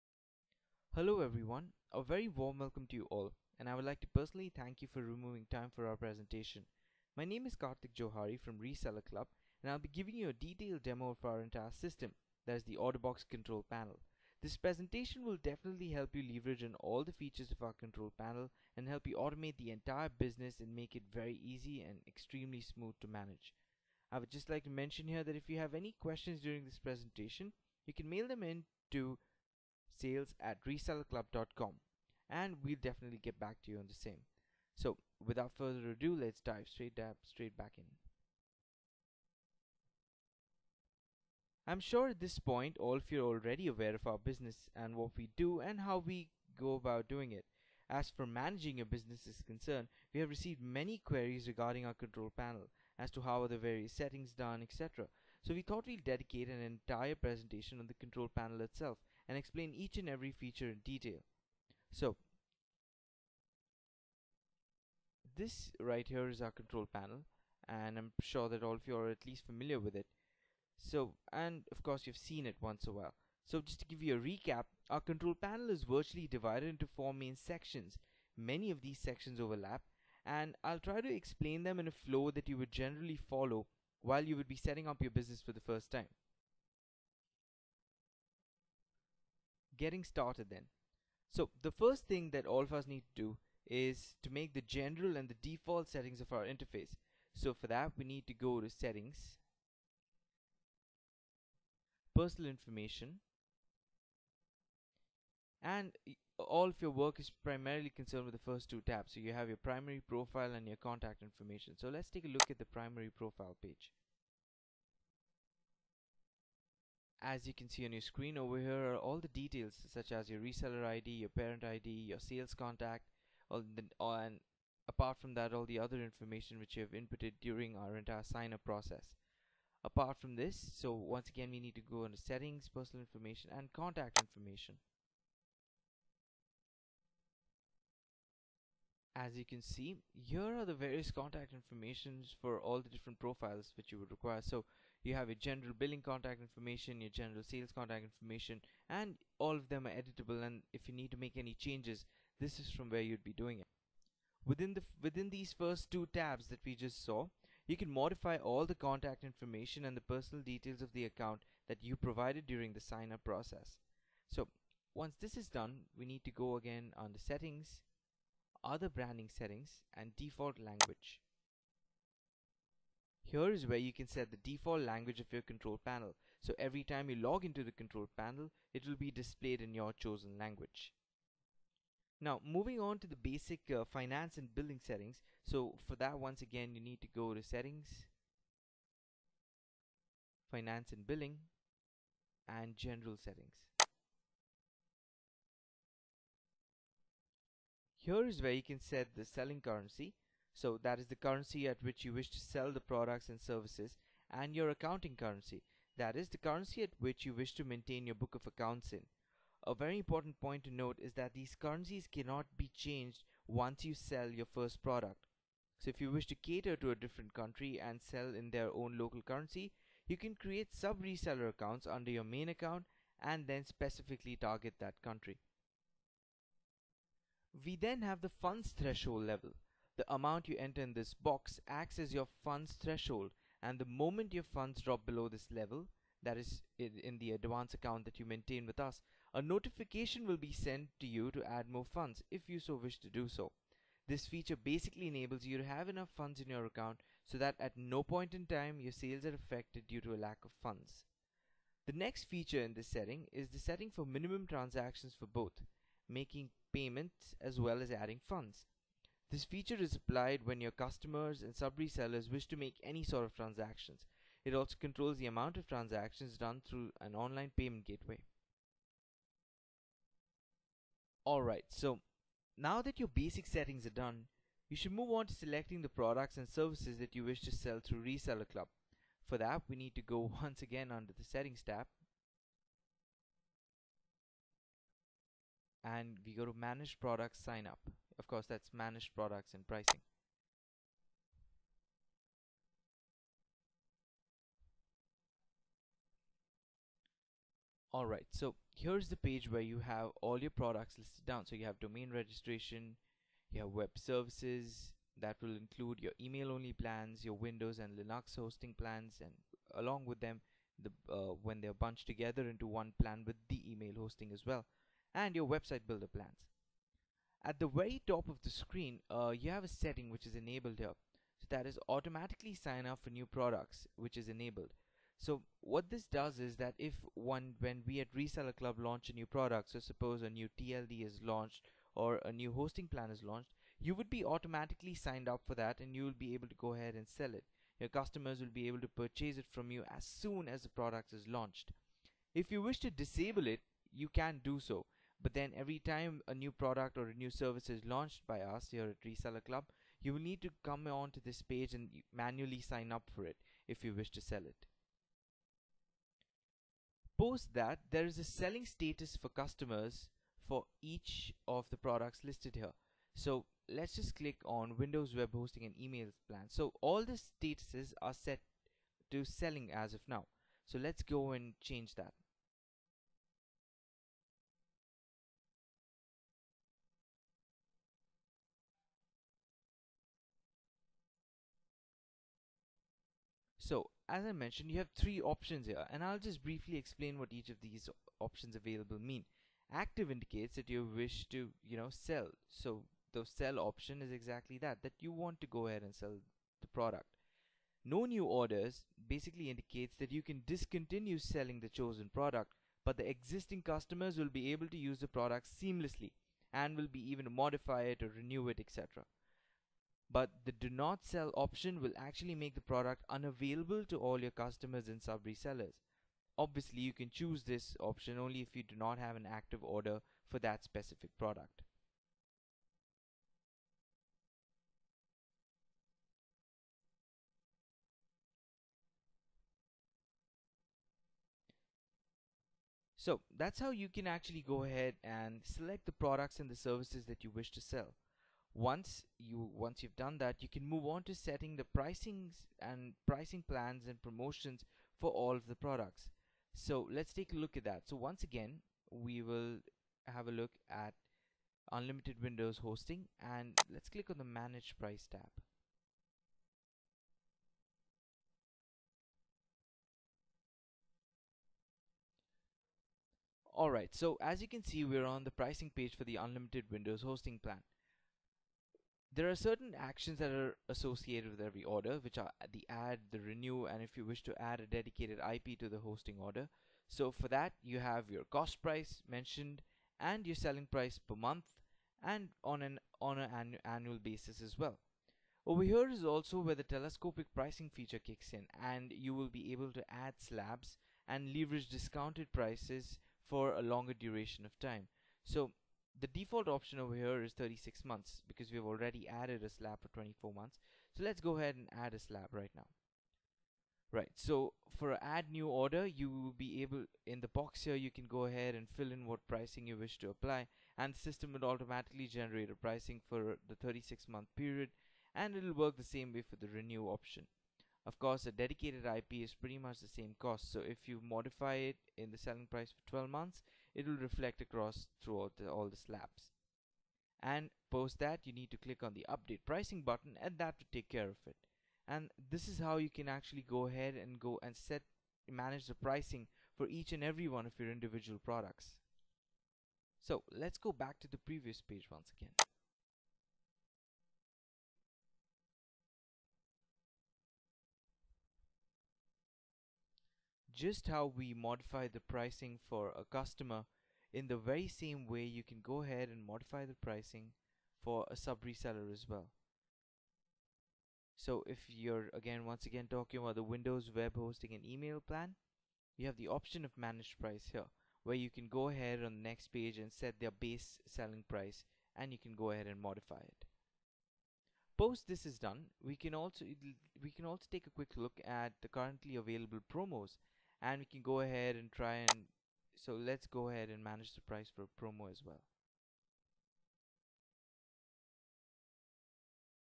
Control Panel Demo Webinar - Part 1
This webinar explains some of the lesser known and yet highly useful features of the ResellerClub Control Panel. It is aimed at empowering you to use the Control Panel to the fullest.